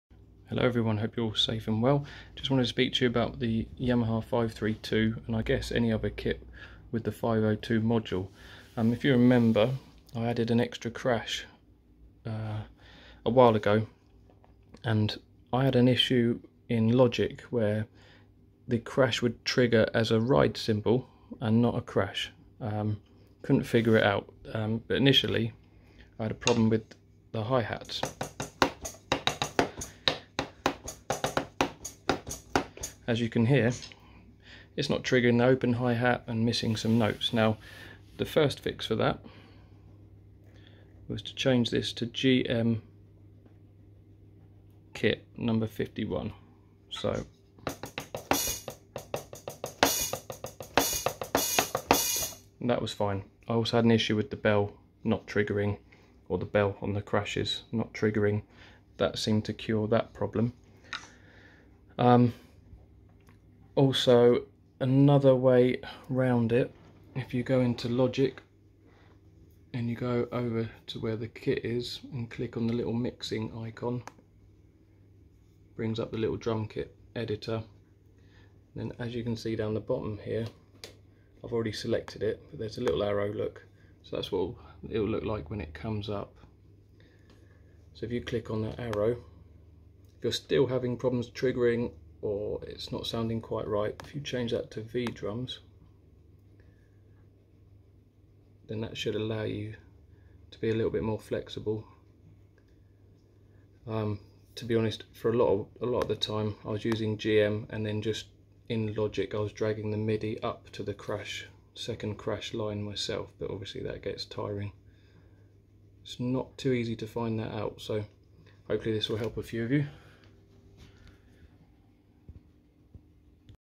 Yamaha DTX 502 triggering issue fix
Hopefully this will help some of you out who are having trouble with triggering hi hats/rides in Logic pro x with a Yamaha 502 module.
I have to thank Louis Sellers and Oli Wiseman for their help in getting it all working.

Please let me know if you've found other ways to achieve the same thing